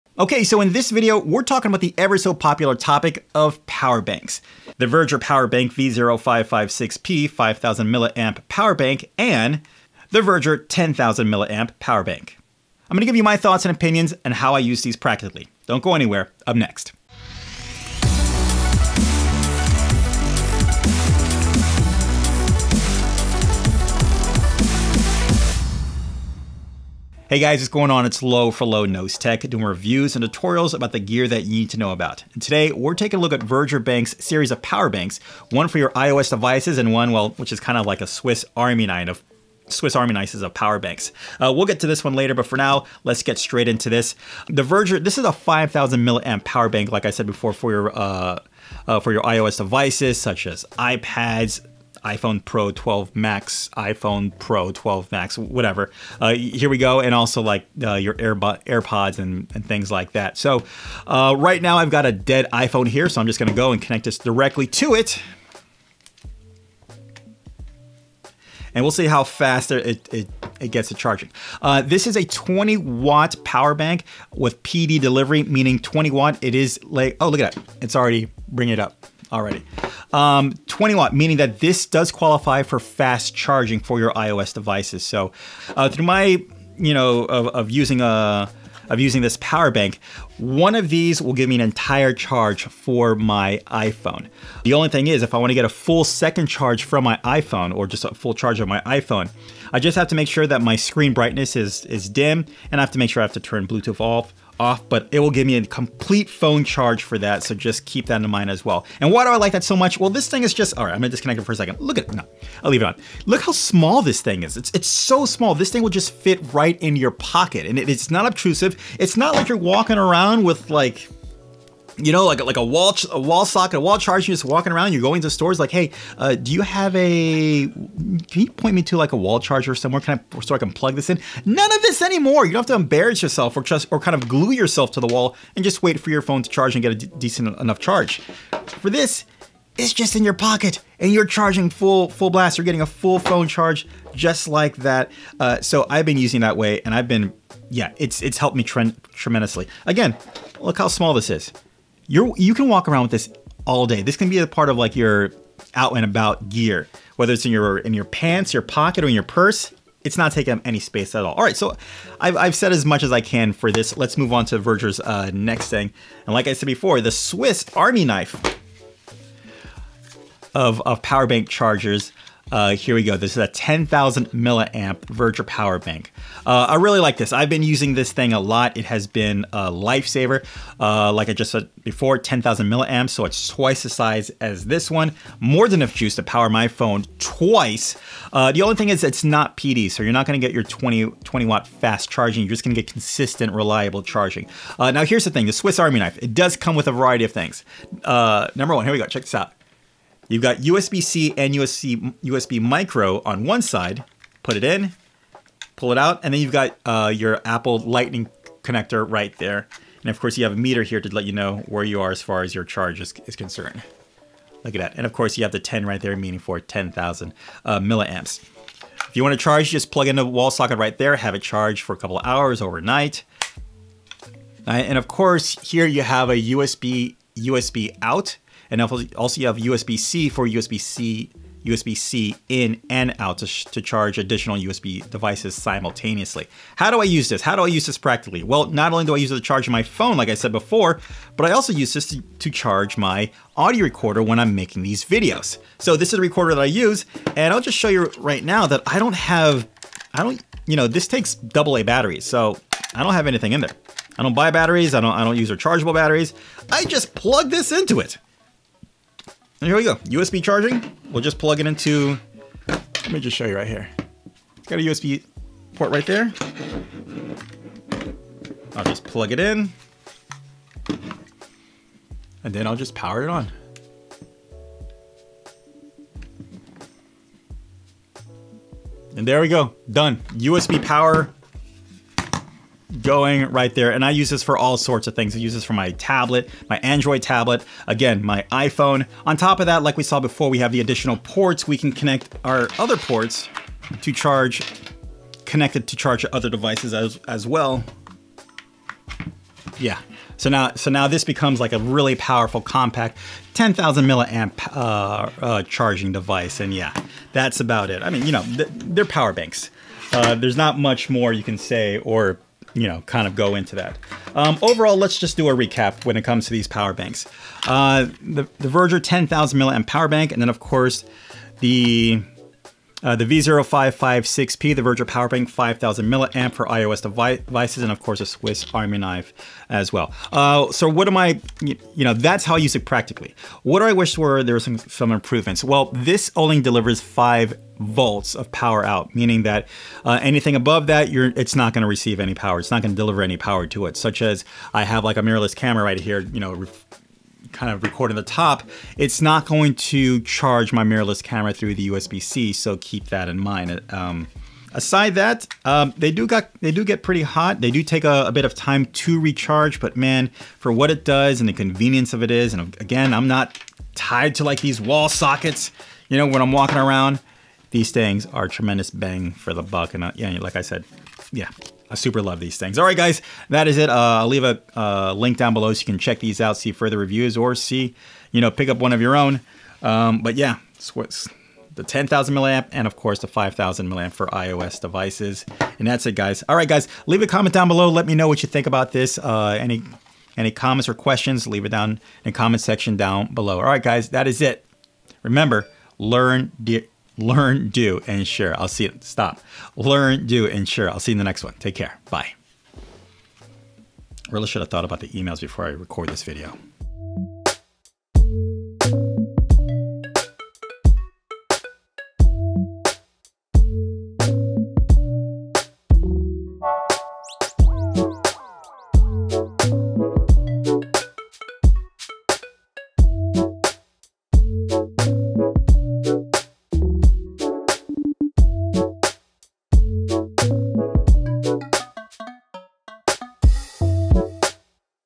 MUST HAVE POWER BANKS | VEGER 5000 and 10000 mAh Power Bank | Review
More power should never be an issue anymore with Vegers V0556P 5000mAh PowerBank for IOS devices and the swiss army knife of power banks the Vegers V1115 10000 mAh PowerBank. Fantastic options to keep your devices charged on the go or in a creative environment. With 20W  + PD for the V0556P and 38W for the V1115, you are going to love it.

Featured Gear:
Veger v0556P (for IOS)
Veger 1115 with built-in cables

(Current Gear)
Camera A
Camera B
Main Microphone
Audio Recorder
RGB Background Light
Best Home Mixer (with bonus stuff)
(Previous Gear)